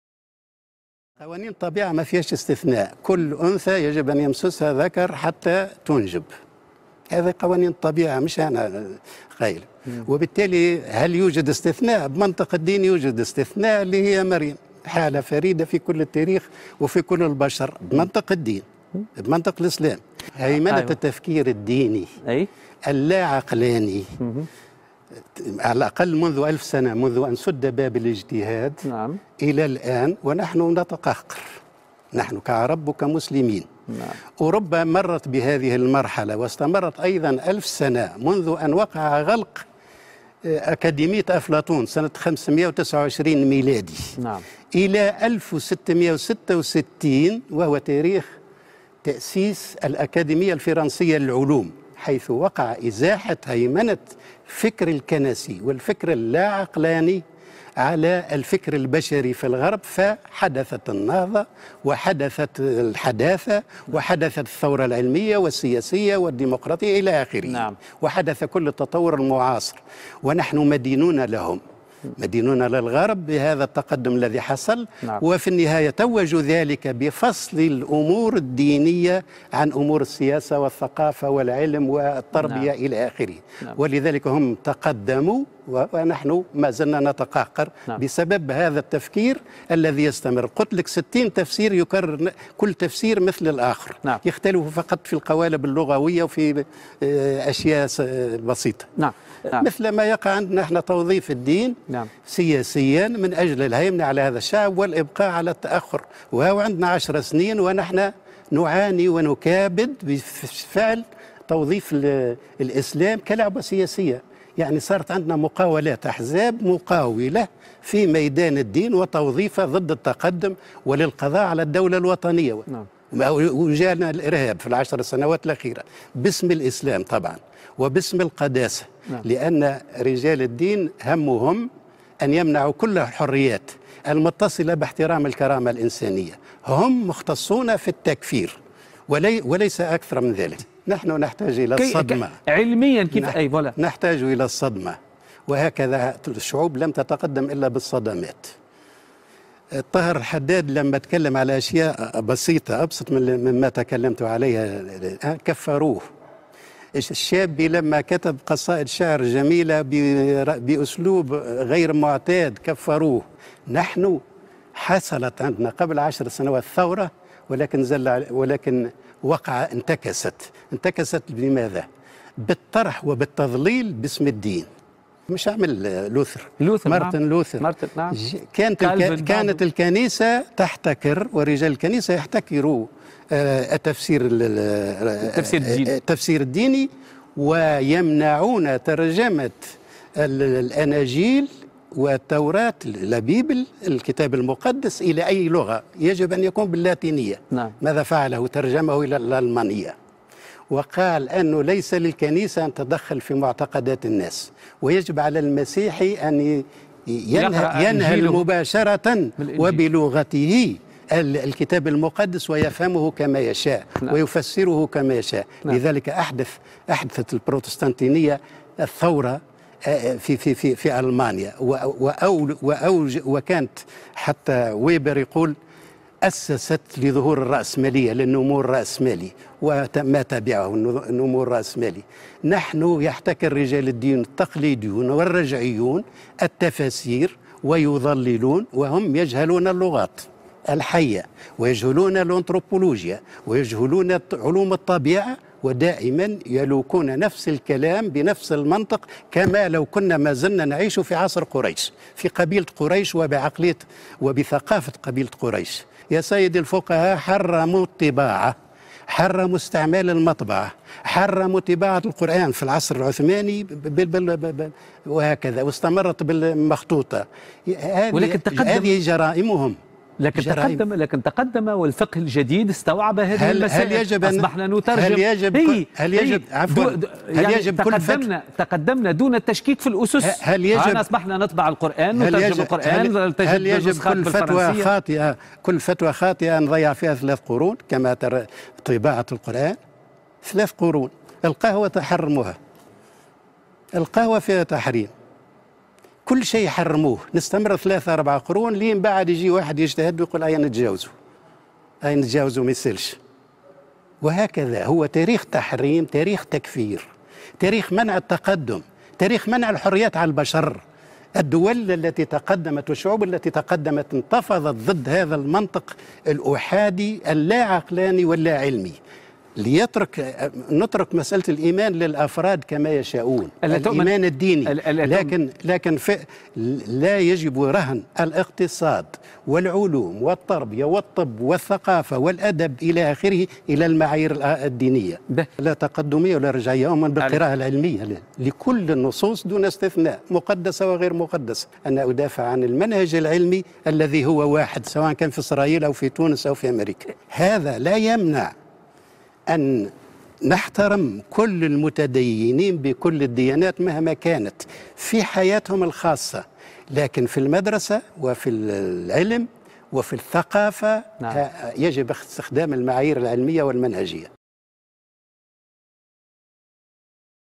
عفيف البوني وكتابه عن القراءة العلميّة للقرآن
Afif Bouni parle de la lecture objective du texte coranique (nouveau livre).
Je fais un extrait de son interview par B. Bsaies.